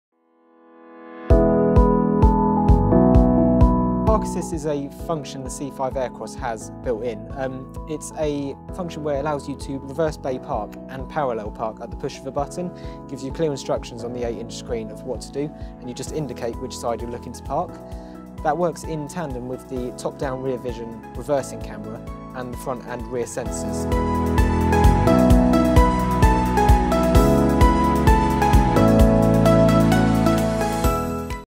Citroen C5 Aircross Park Assist and Vision 360
Parking is never easier on the new Citroen C5 Aircross. With Park Assist, the vehicle can detect a suitable space and will automatically reverse into a parking bay, using its 360-degree Top Vision view and front & rear camera systems. You can even automatically parallel park in this model. This feature is available as an option.